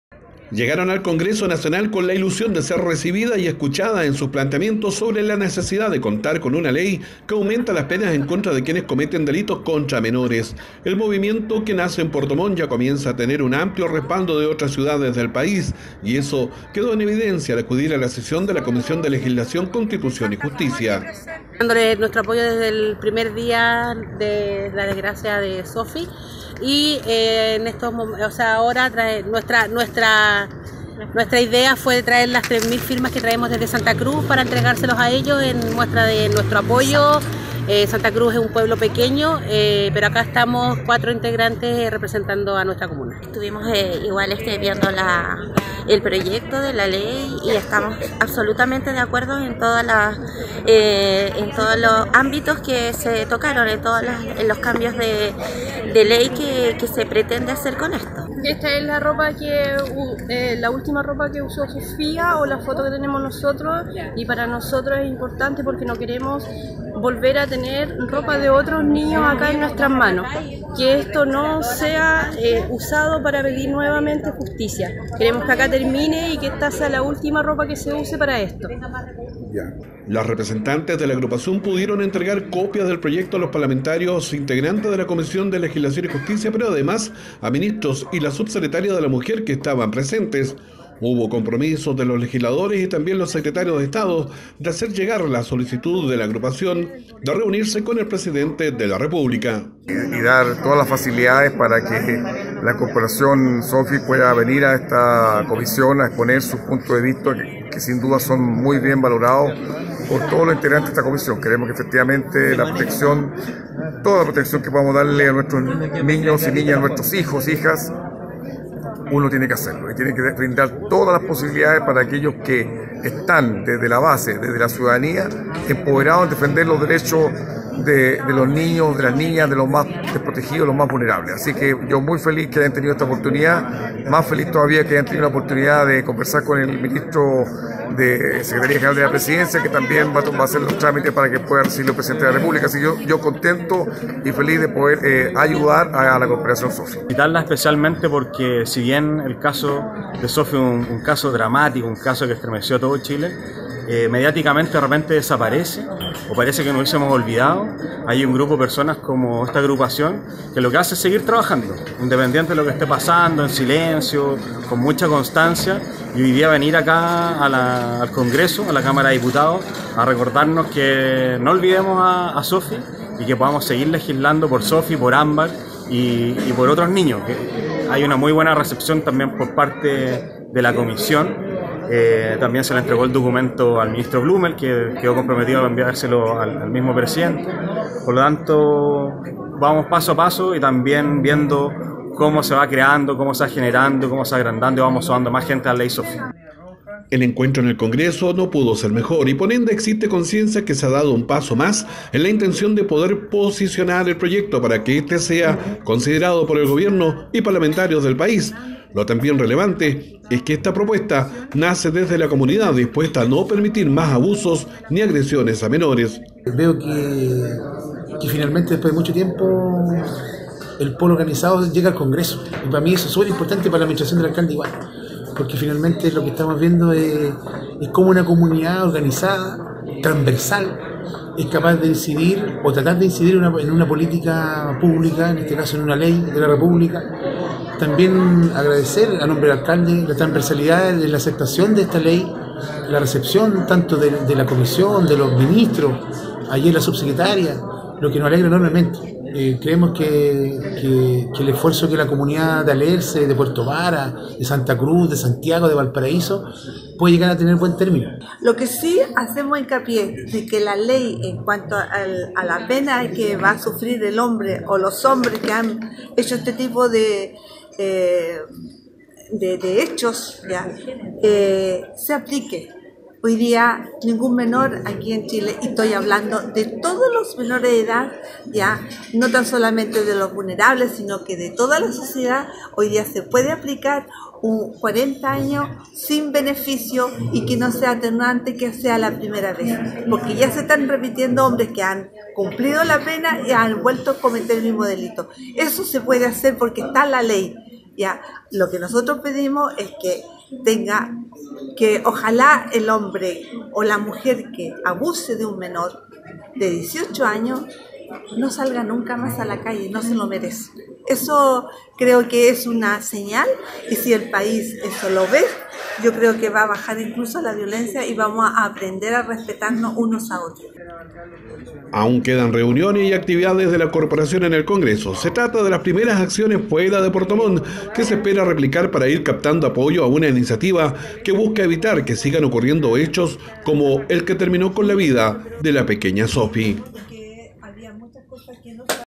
Corporación ley Sophie chile, entrego a parlamentarios y ministros el proyecto de ley
Corporación ley Sophie Chile, entregó a parlamentarios y ministros el proyecto de ley que busca aumentar las pena contra quienes cometen abusos y maltrato a menores